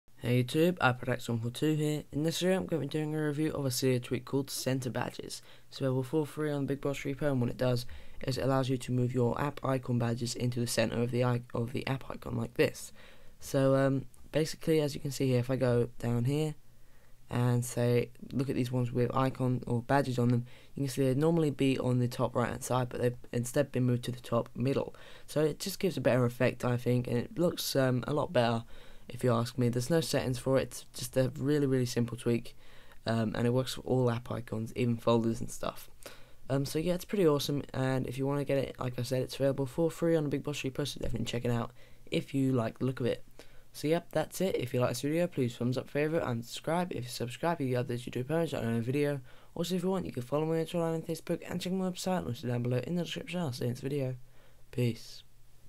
Move App Badges To The Center | CenterBadge Cydia Tweak Review
Name: CenterBadge
Price: Free!
Repo: BigBoss
Developer: Jonathan Winger Lang

Leave a comment or PM me if you have any questions.
Need help with anything iOS, mac, windows or android? Then message your problems/questions to @iSupportiOS on twitter. Expect a fast reply!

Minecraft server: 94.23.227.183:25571